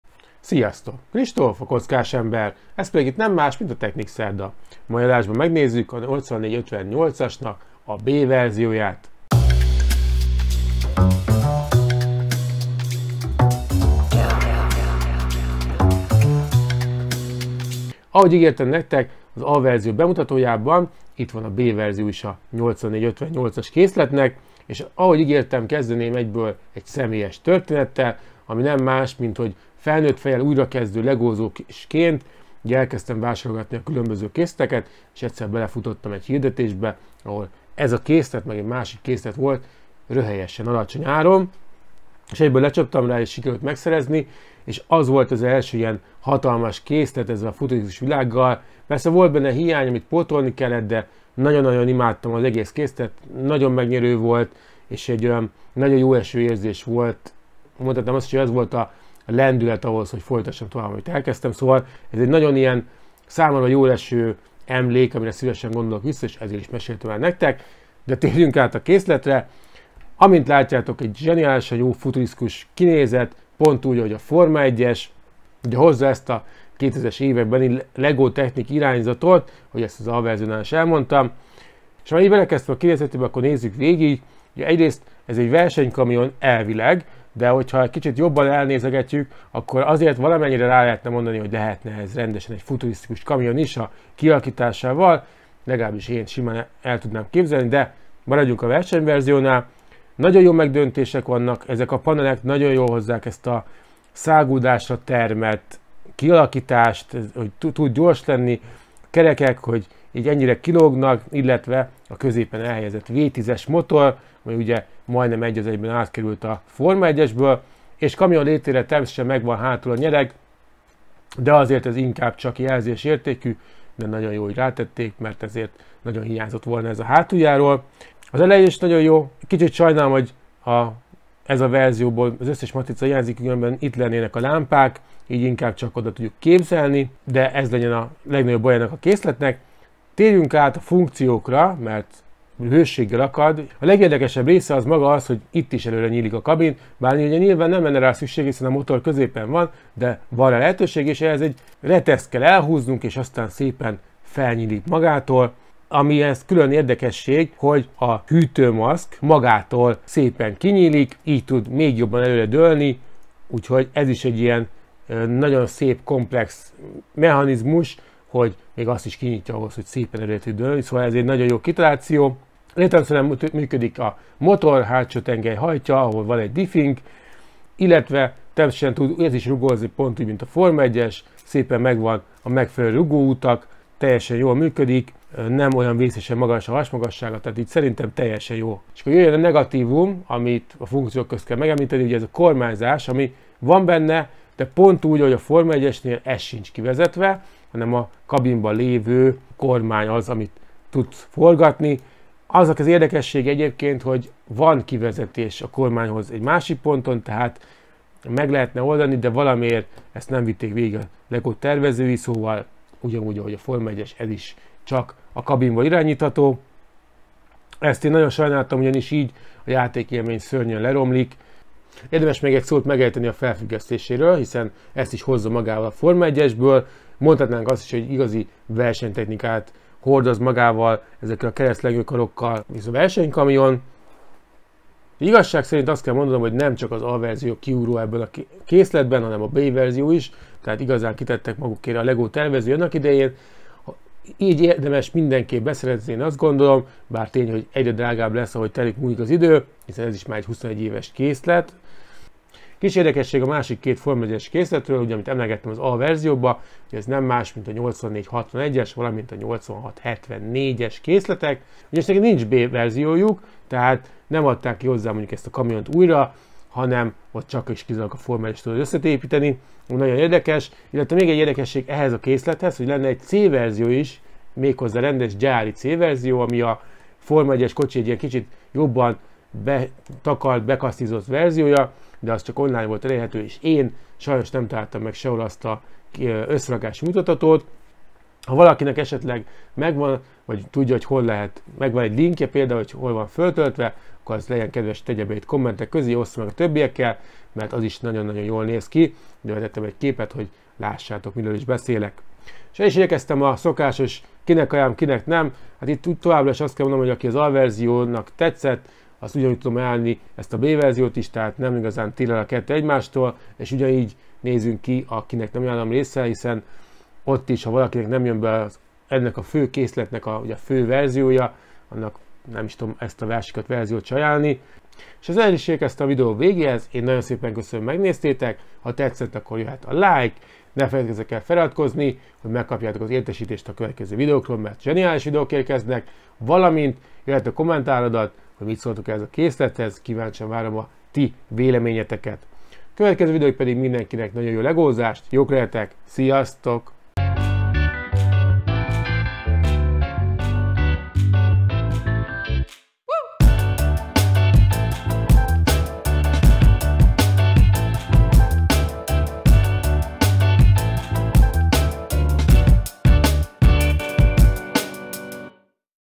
Technic szerda - A versenykamion - 8458 B modell
A Lego Technic 8458-as készlet A modellje is hihetetlen volt, de ez a B modell rátesz még egy lapáttal. Nem lehet nem szeretni ezt a készletet!

Itt tudod megnézni a pólókat. Ha valamelyik tetszik, akkor írj rám!